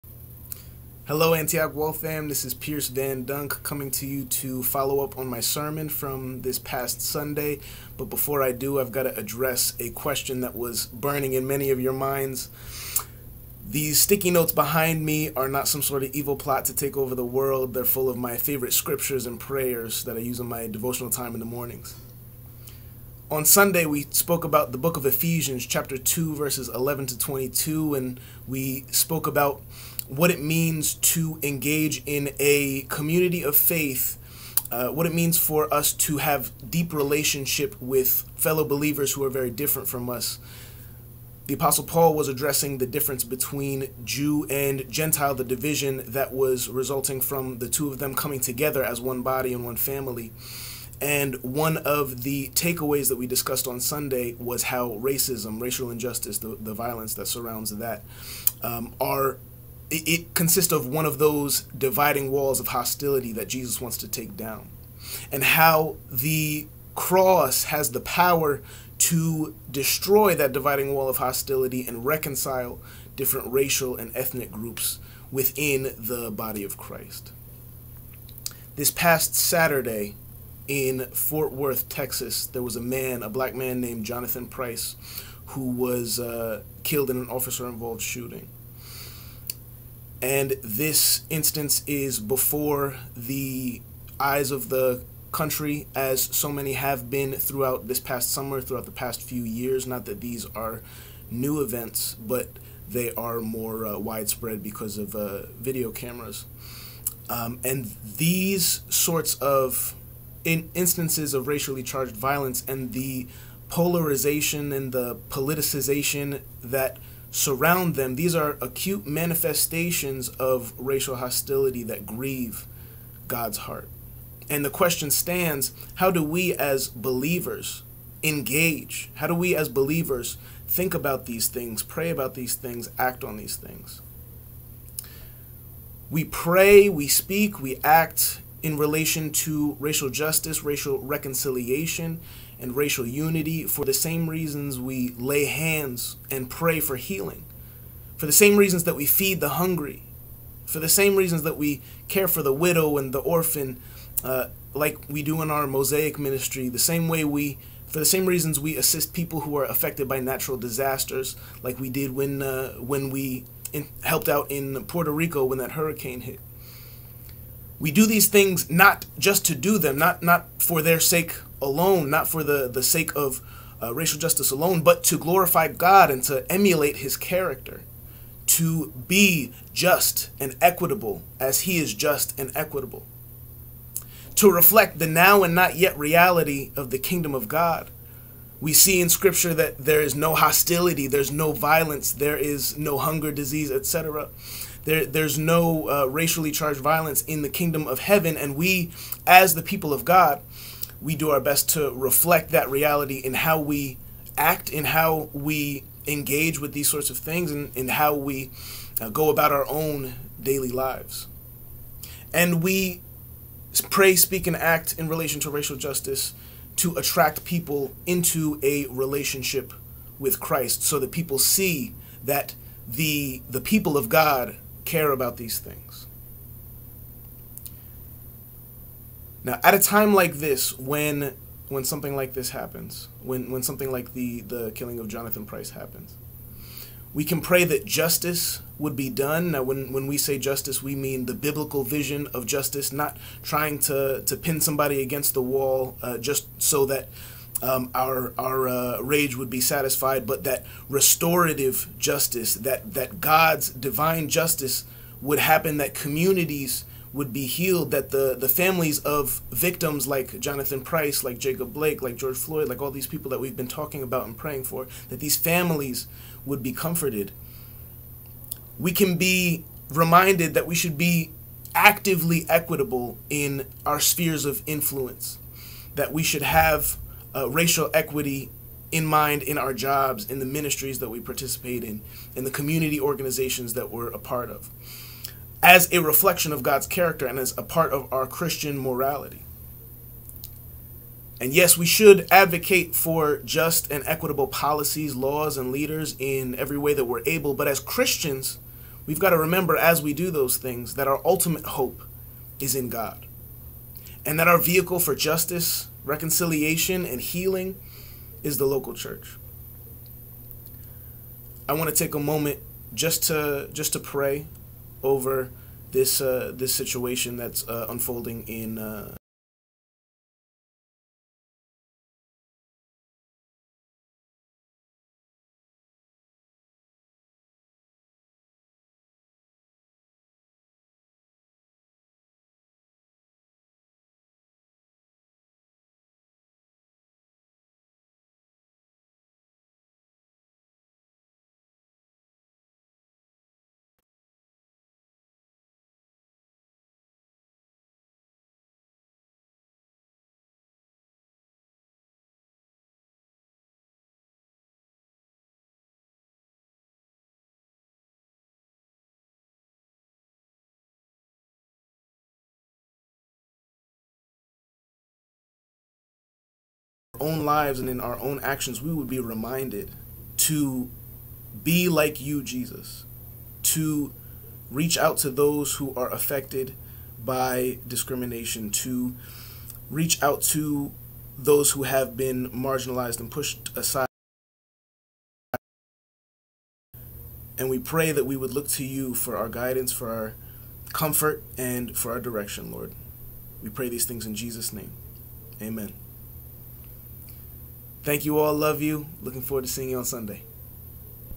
A Midweek Message from Pierce VanDunk: Racial Injustice as a Barrier to Christian Unity
Pierce VanDunk follows up on his Ephesians 2:11-22 sermon by looking at racial injustice as a barrier to Christian unity. He focuses on the recent killing of Jonathan Price.